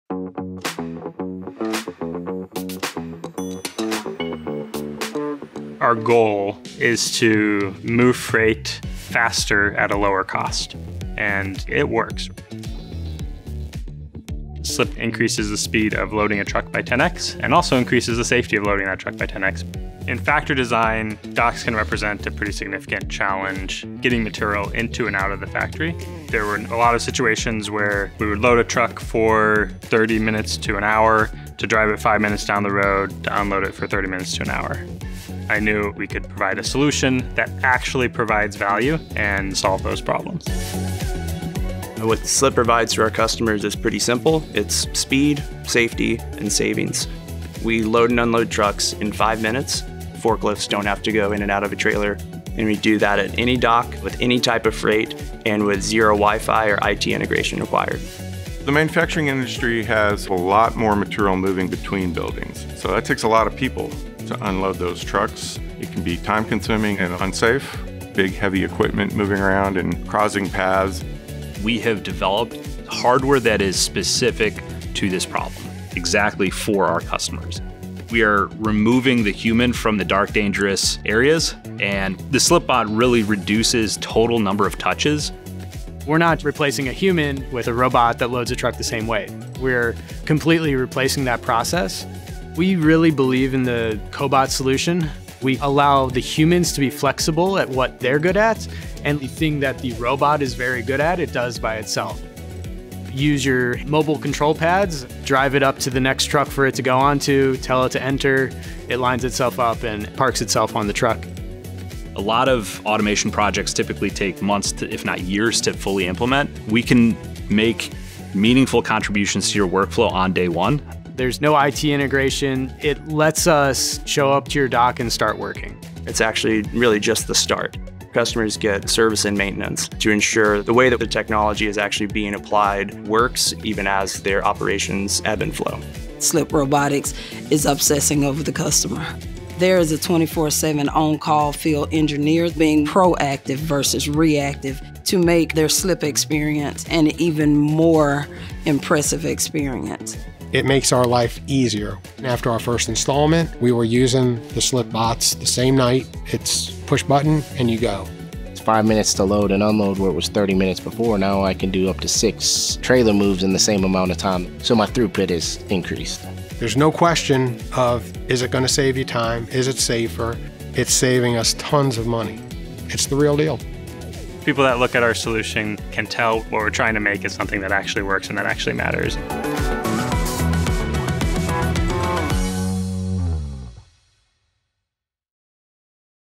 Slip Robotics: Auto-load/unload any truck in 5 minutes
🚛 What if you could move freight 10x faster and 10x safer— today?

At Slip Robotics, we’re redefining dock operations with speed, safety, and savings. SlipBots eliminate time-consuming, labor-intensive, and risky bottlenecks with automation that works on day one.

⚡️ Speed: 5 minutes to load/unload all types of freight (floor-loaded boxes, pallets, crates, etc.). 10x faster than the status quo.

👷 Safety: 0 forklifts or people rushing in and out of trailers. 10x safer than the status quo.

✅ Simplicity: 0 Wi-Fi or IT integration required, and no need for custom trailers, dock modifications, or other infrastructure changes. 10x simpler than the robotics implementation status quo.

🎥 Hear directly from our team and customers how we are transforming the way freight moves.